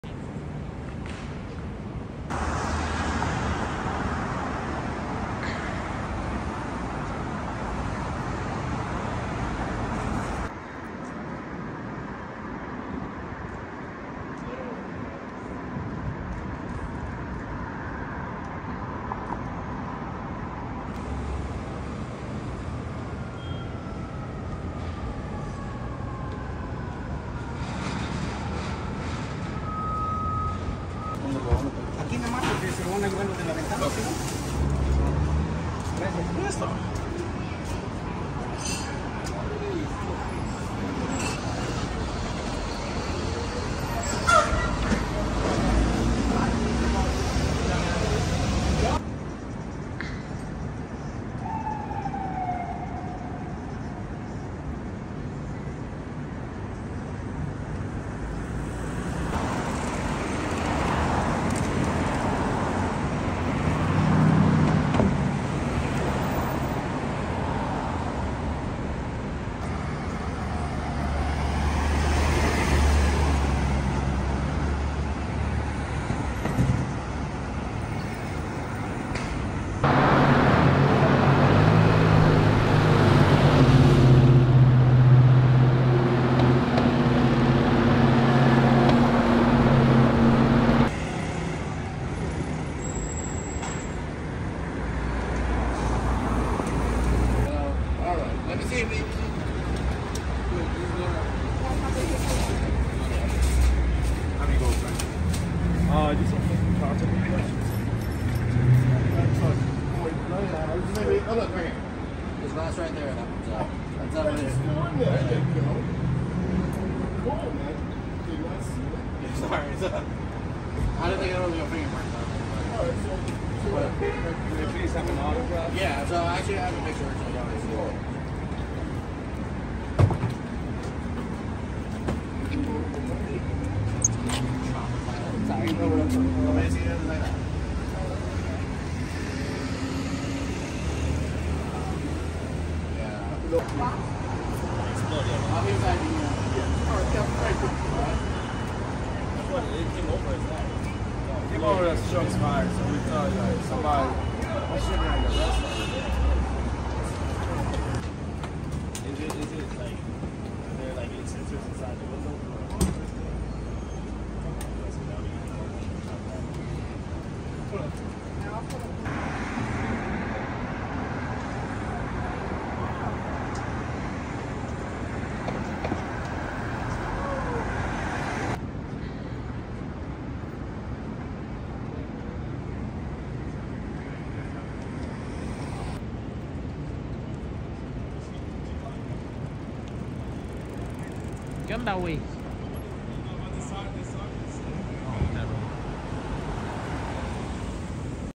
Police investigation at VEZZO Italian restaurant, Window Broken 9.8.24
Video By Devin_VNP

Videojournalist | Stringer | 新聞播報員| 社区信息传播者| Breaking NYC News Coverage. Crimes , Fire, Crashes . Viral Clips.